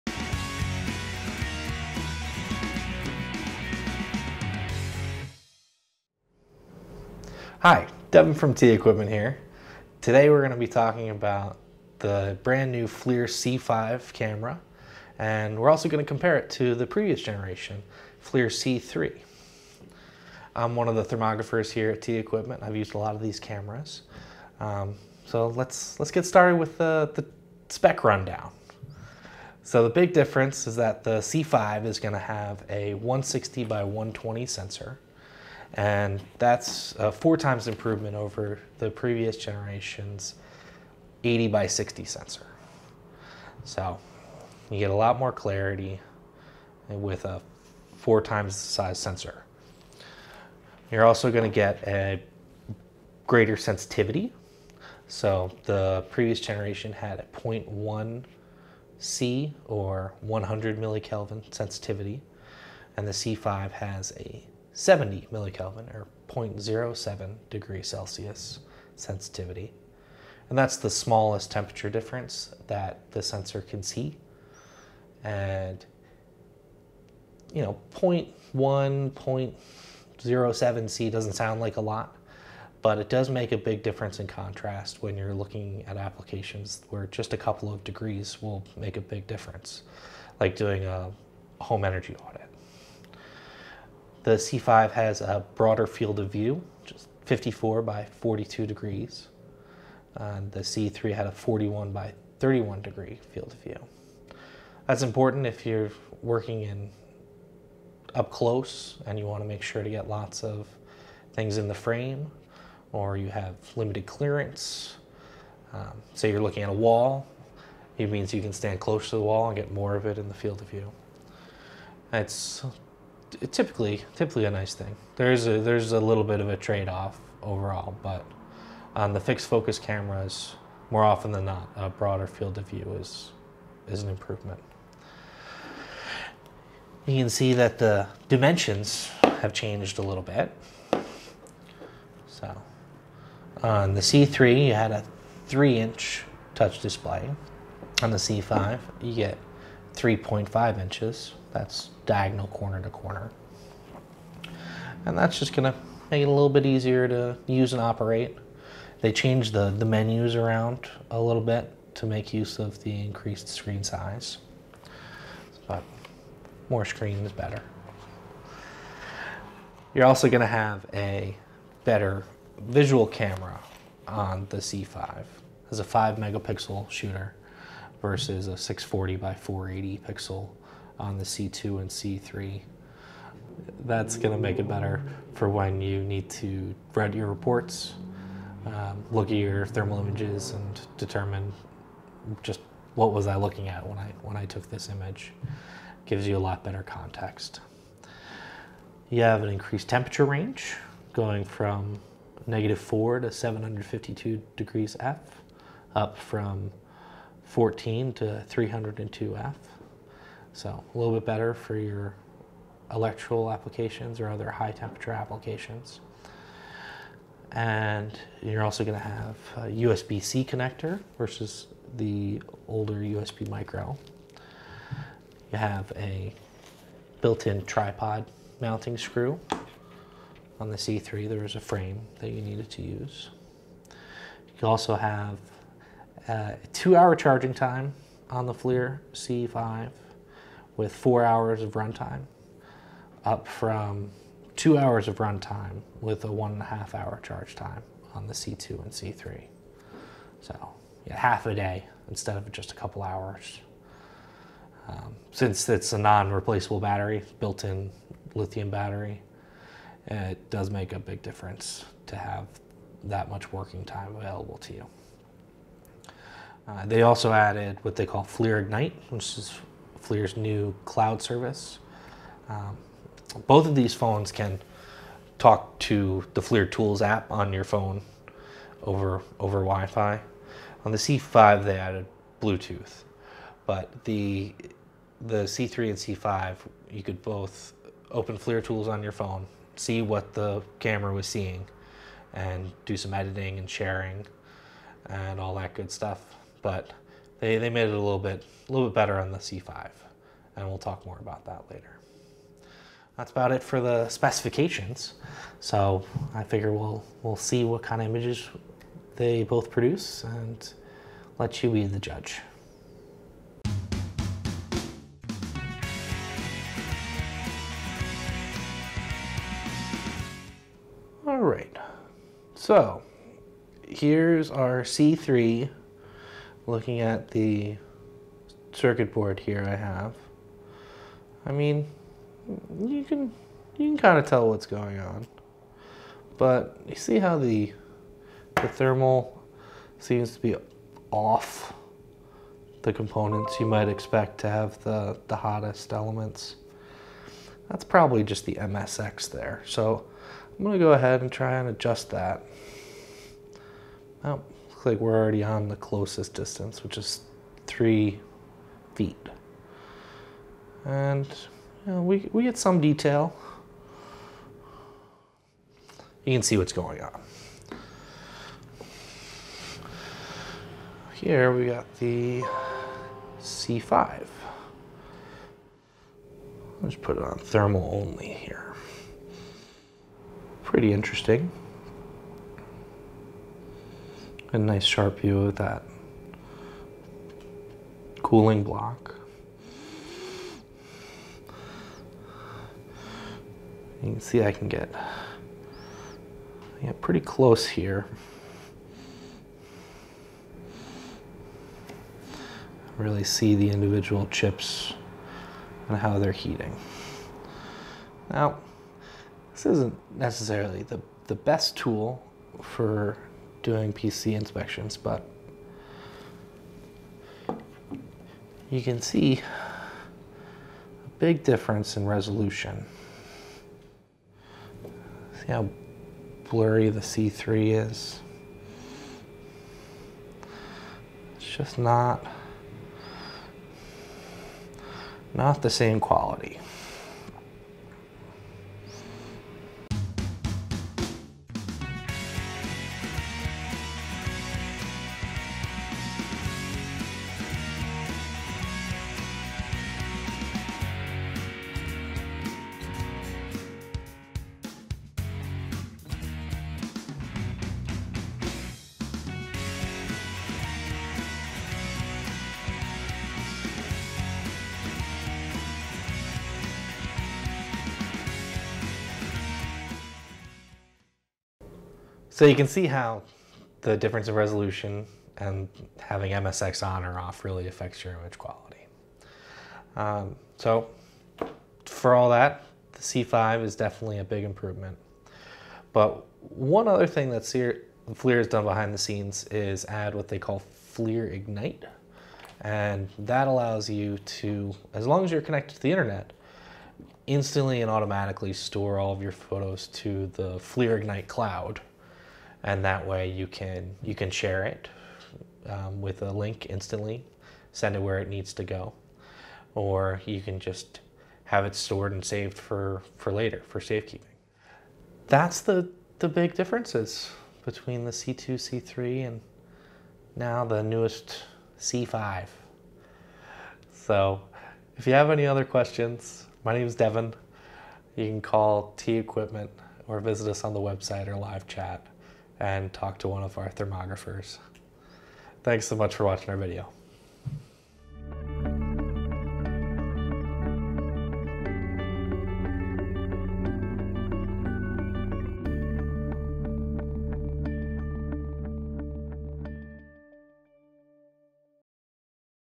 FLIR C5 vs C3 Thermal Camera Comparison
Specifications: 00:25
Live Demo: 05:15
Sample Images: 07:21
FLIR Ignite Cloud: 08:01

In this video, TEquipment staff thermographer Devin takes a close look at the big differences between the new FLIR C5 and the old FLIR C3 thermal cameras. Both are pocket-sized thermal cameras, but the C5 packs much higher IR resolution, better sensitivity, a wider temperature range, and new features like FLIR Ignite Cloud.

The C5 packs 160 x 120 thermal imaging power in a rugged, pocket-sized package. It includes FLIR MSX technology for extra visual details, and will save you time on the job with 1-Touch Level/Span auto adjustment.

The C5 also lets you upload images directly to the FLIR Ignite cloud so you can easily store, organize, and back-up images. You can then quickly add annotations, securely share findings, and even create professional reports.

The C5 is a professional-grade tool, rated to IP54 against dust and water and tested to withstand a 2 meter (6.6ft) drop. It can easily fit in your pocket since it's smaller than a smartphone, and has a bright built-in LED flashlight.